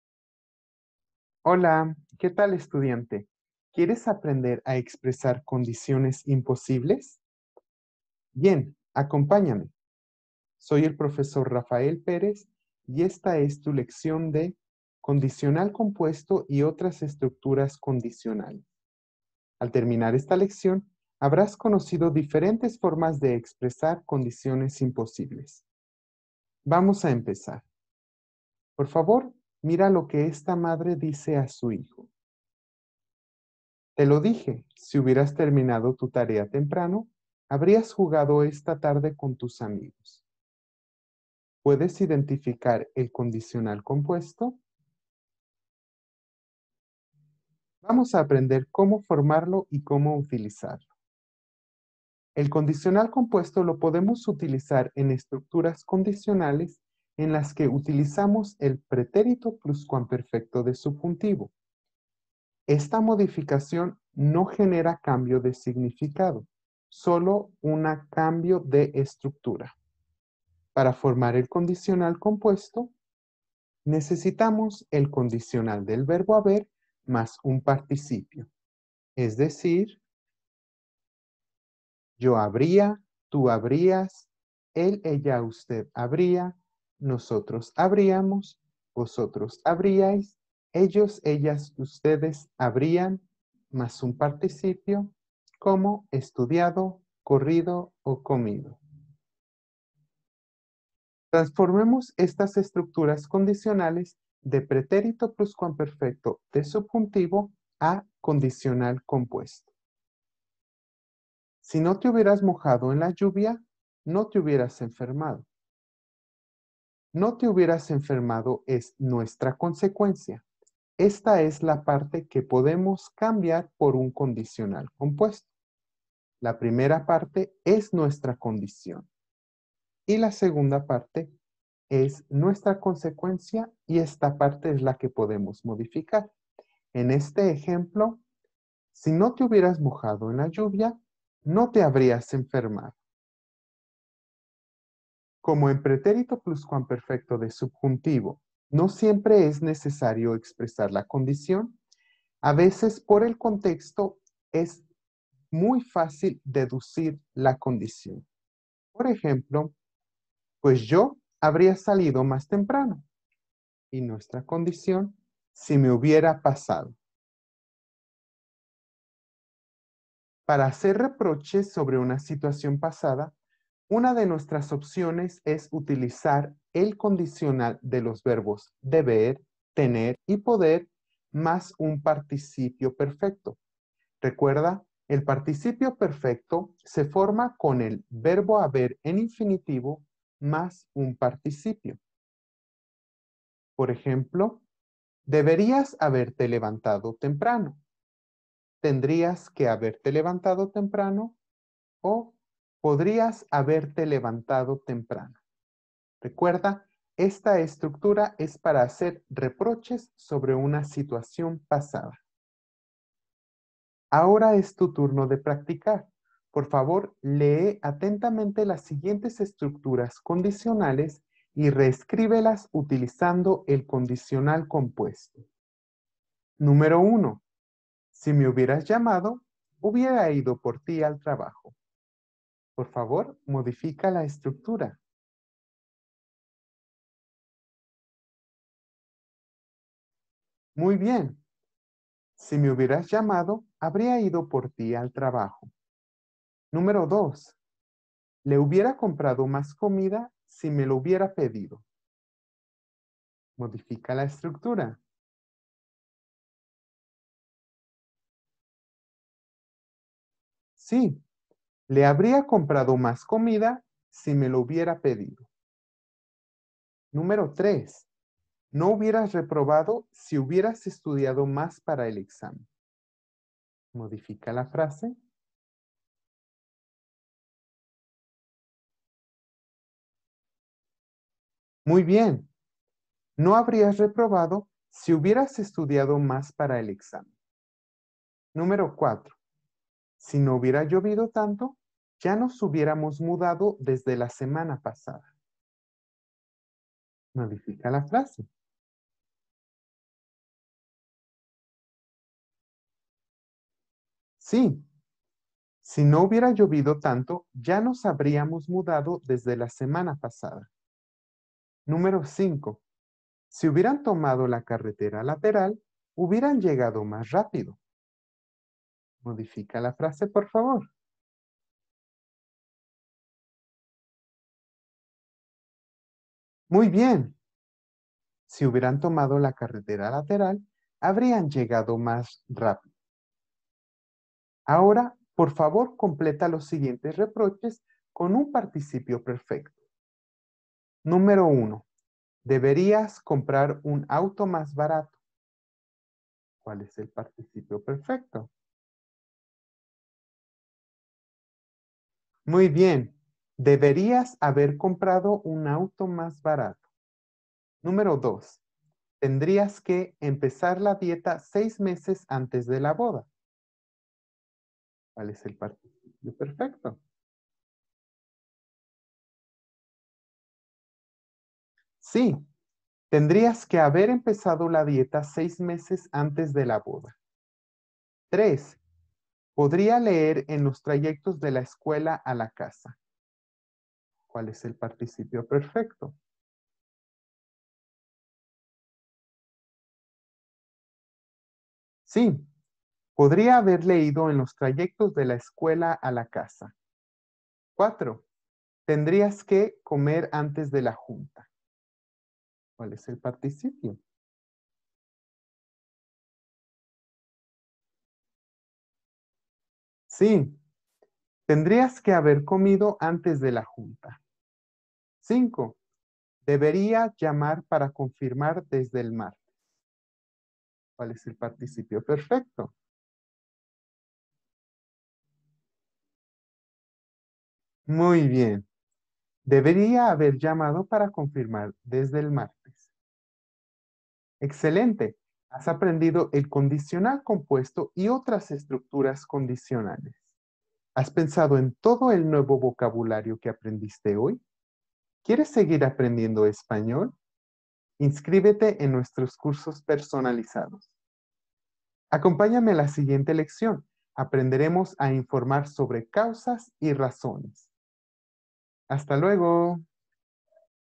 Learn Spanish: Composite conditional tense in Spanish.
This lesson is for the compound conditional and use in Spanish.